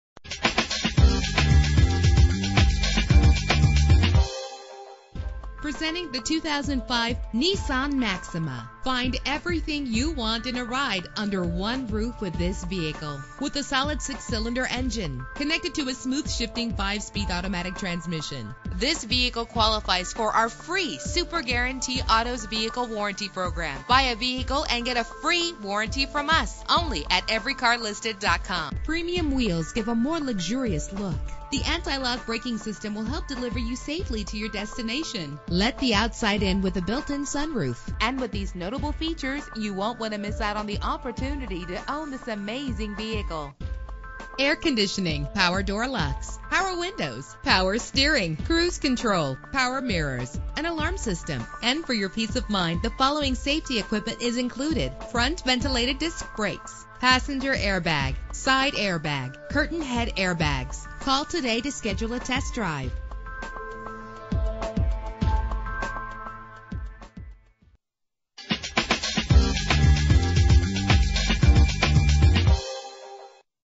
2005 Nissan Maxima - Woodstock GA
Year: 2005
Make: Nissan
Model: Maxima
Trim: 3.5 SE
Engine: 3.5 liter 6 cylinder 24 valve
Transmission: 5-Speed Automatic
Color: Gray
Mileage: 109409
Address:
7871 Highway 92
Woodsto